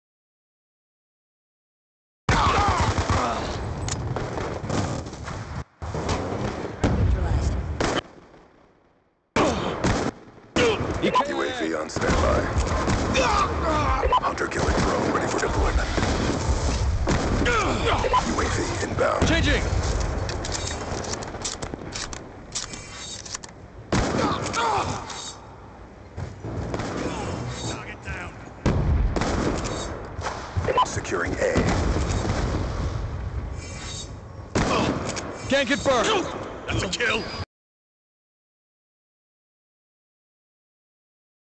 Stolen Remington
I picked up a kids remington i found and then got a bunch of kills in their spawn... it was really fun